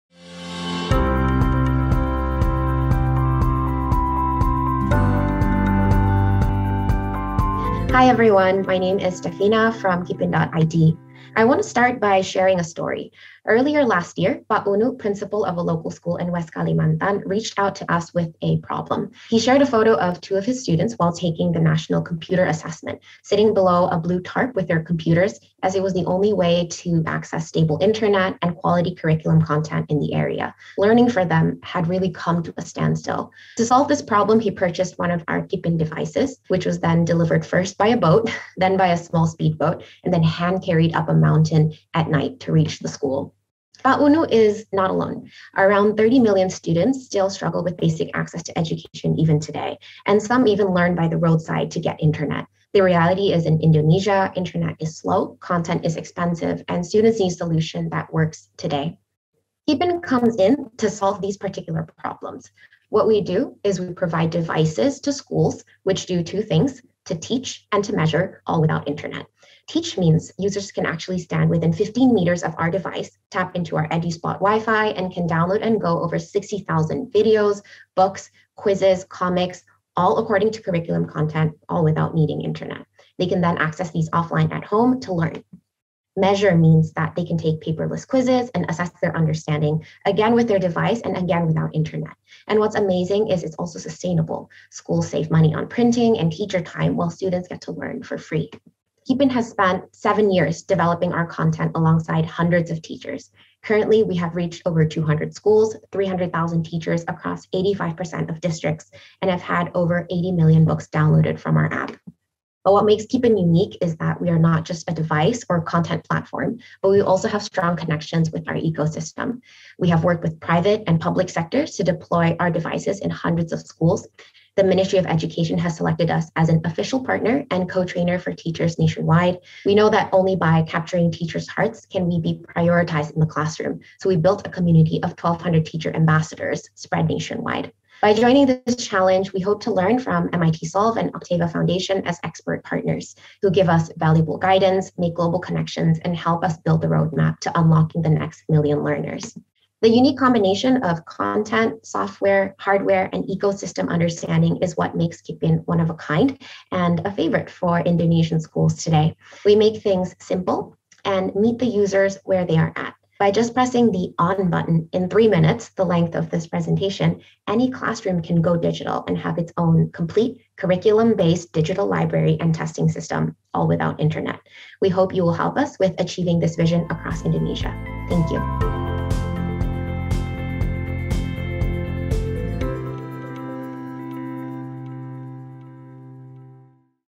Kipin Selected as Winner of Global Competition: MIT x Octava Foundation Social Innovation Challenge.
Kipin.id has been officially announced as a winner for Octava Foundation's Social Innovation Challenge, powered by MIT Solve and supported by UBS Optimus Foundation.

Over 200 solutions were submitted globally and 18 judges across leading education venture capital firms, impact funds, foundations and education experts evaluated these solutions to arrive at 10 global finalists, along with support from MIT Solve. Applicants were asked to undergo a rigorous selection process including an extensive written application, several pitch recordings as well as a live interview with over 12 education experts and judges globally. Solutions were judged based on 8 key criteria: alignment, potential for impact, feasibility, innovative approach, leadership ability, human centred design & inclusion, scalability and partnership potential.

The Social Innovation Challenge is a prestigious global competition that seeks to find edtech solutions which can improve learning outcomes for underserved learners in Southeast Asia, while being accessible and affordable. The challenge is a collaboration between MIT Solve, an initiative of the Massachusetts Institute of Technology (MIT) with a mission to drive innovation to solve world challenges, and Octava Foundation, a Singapore based private grant maker focused on enabling equitable education. This challenge is supported as well by UBS Optimus Foundation.

Kipin is proud to be selected as one of 10 winners globally, identifying it as one of the top edtech solutions serving Southeast Asia's underserved learners. VP of Business Development Steffina Yuli, said, "We are honoured to be selected as a winner and very excited to join MIT and Octava Foundation's network of partners. MIT is a top technology university globally and Octava Foundation is a leading education thought leader in the region. This brings us to a global stage. Together, this will help to bring Kipin's solutions to even more learners nationwide, and beyond."

As a winner of this challenge, Kipin will receive prize funding as well as a ten-month tailored support program run by MIT Solve and the Octava Foundation, which provides close mentorship, customised support and access to MIT and Octava's extensive network of partners.

To learn more about Kipin's solution, please visit:
- Full application: